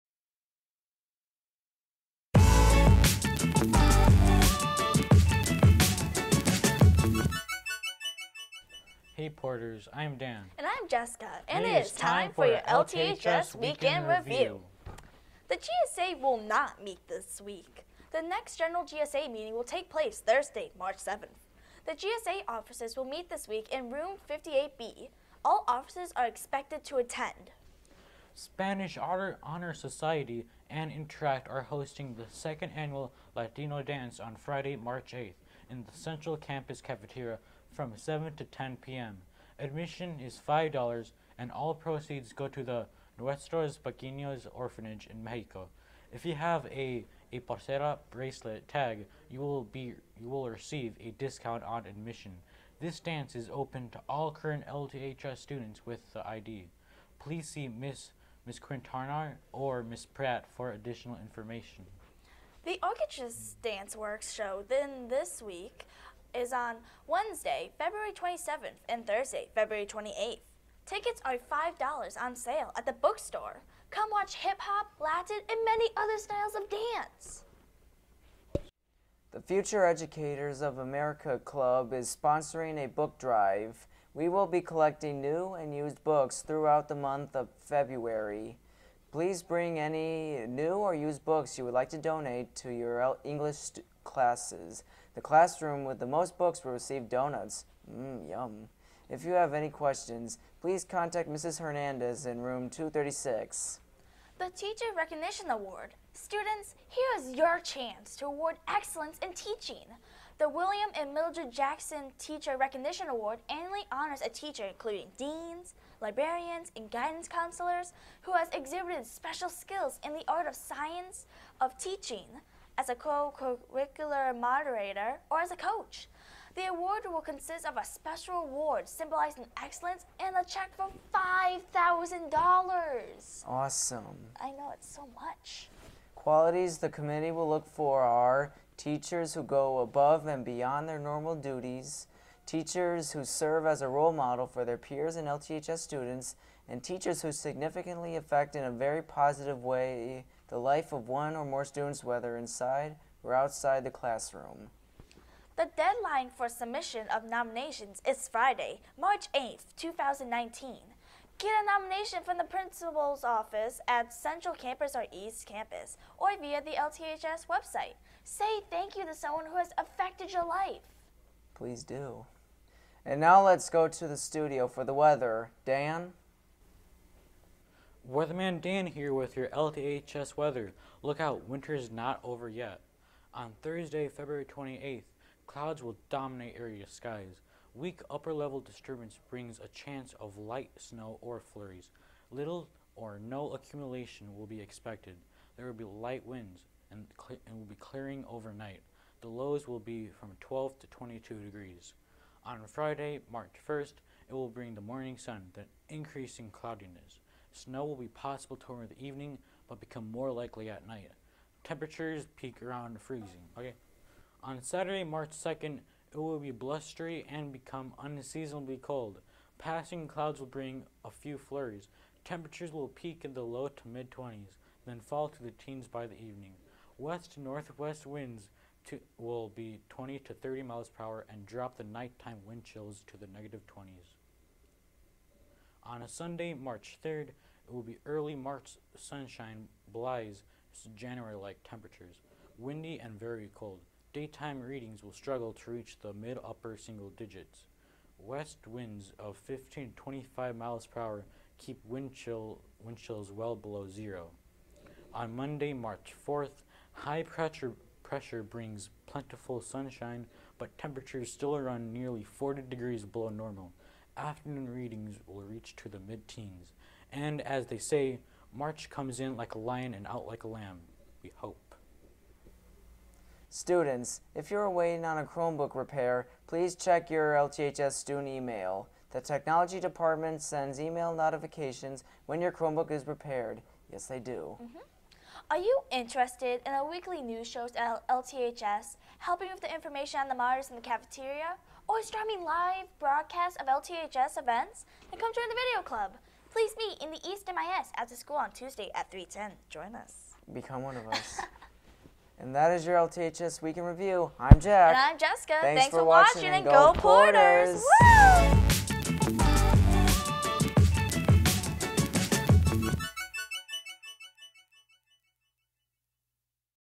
LTHS Week In Review February 28th, 2019
Check out this week's edition of Week In Review for the Porters.  GSA meeting info, Latin Dance Night,  Orchesis Danceworks, Book Drive, Weather man Dan, Don't forget to check your school email if your Chromebook is getting fixed.  This is all part of this week's LTHS Week In Review!  Join Video Club!